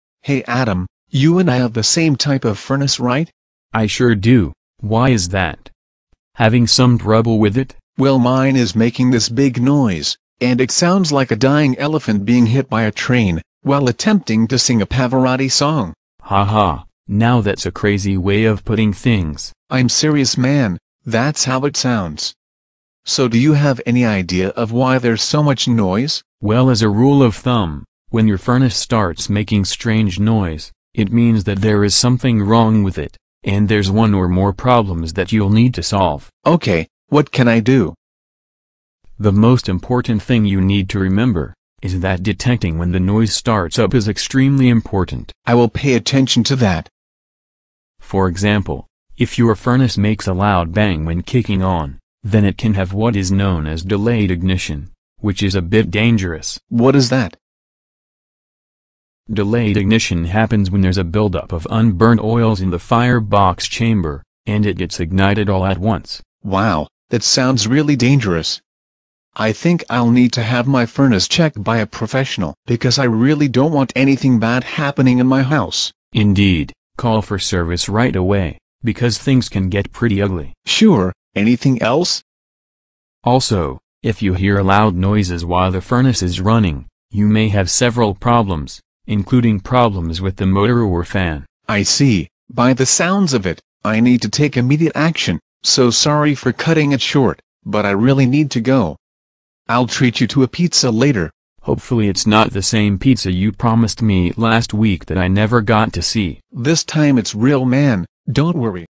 Why is My Furnace So Loud?
My furnace makes so much noise I cannot even hear myself think! Why is my furnace so loud? Is there any way I can get rid of, or at least reduce the noise?

Hey Adam, do you have one of those furnaces that I got last week, too?

I sure do Miles, why is that? Having some trouble with them?

Well mine is making this big noise and it sounds like a dying elephant being hit by a train while attempting to sing a Pavarotti song.

Haha, now that's some crazy way of putting things.

I'm serious man, that's how it sounds. So do you have any idea of why there's so much noise?

Well as a rule of the thumb, when your furnace starts making strange noise, it means that there is something wrong with it and there's one or more problems that you'll need to solve.

OK...

The most important thing you need to remember is that detecting when the noise starts up is extremely important.

Hm...

For example, if your furnace makes a loud bang when kicking on, then it can have what is known as delayed ignition which is a bit dangerous.

Delayed ignition happens when there's a buildup of unburned oils in the fire box chamber and it gets ignited all at once.

Wow, that sounds really dangerous. I think I'll need to have my furnace check by a professional, because I really don't want anything bad happening in my house.

Indeed, call for service right away, because things can get pretty ugly.

Sure, anything else?

Also, if you hear loud noises while the furnace is running, you may have several problems, including problems of the motor or fan.

I see, by the sounds of it, I need to take immediate action, so sorry for cutting it short, but I really need to go. I'll treat you to a pizza later.

Hopefully it's not the same pizza you promised me last week that I never got to see.

This time it's real man, don't worry!